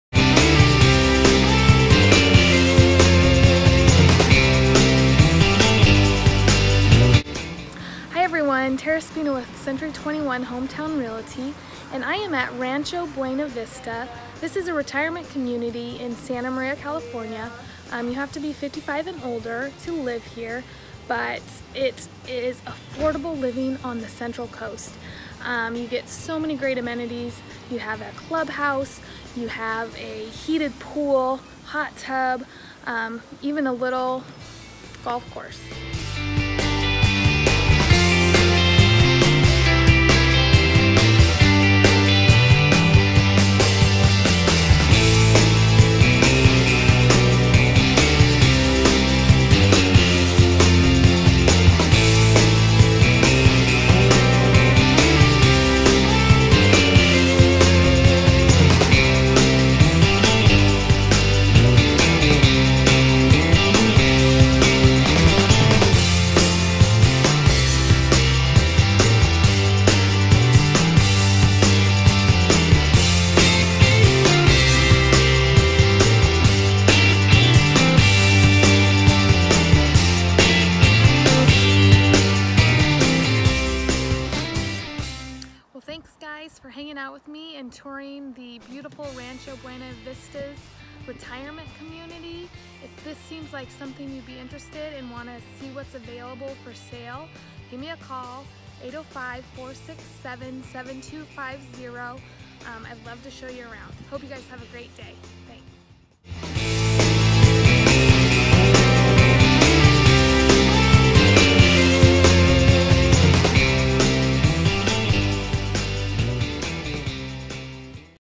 Rancho Buena Vista Mobile Home Park
Rancho Buena Vista Mobile Home Park is a senior mobile home parks located in Santa Maria California. If you would like more information on homes available here give Terra Spino with Century 21 Hometown Realty a call at or email
DRE 02070370